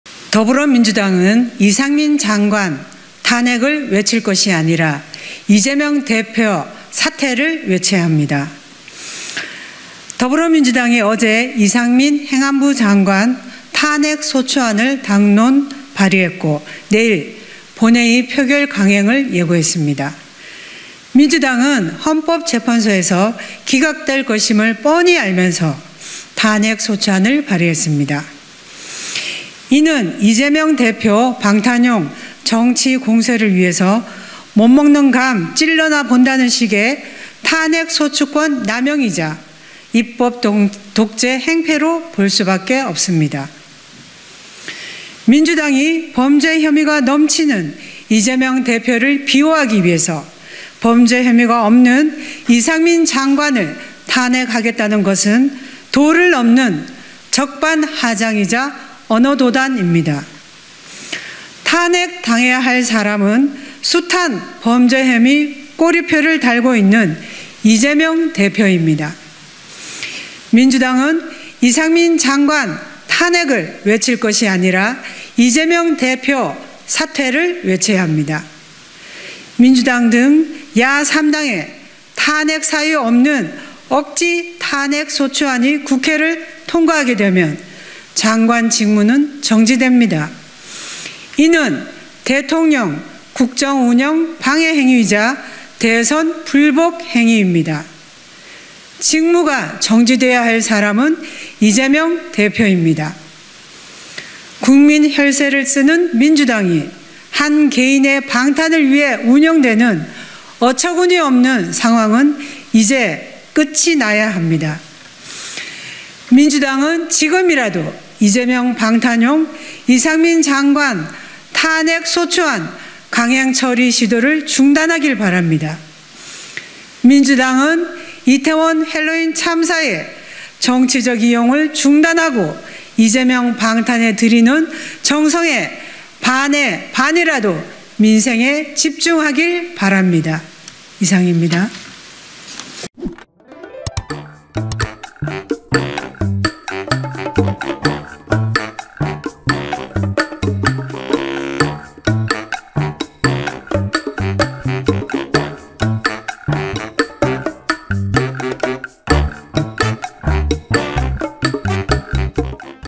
이상민 탄핵 발의한 민주당에 김미애 "이재명 방탄용... 강행 처리 수도 중단하길"
국민의힘 김미애 원내대변인이 23년 2월 7일 국회 소통관 기자회견장에서 브리핑을 하고 있다.

촬영편집.권민구 기자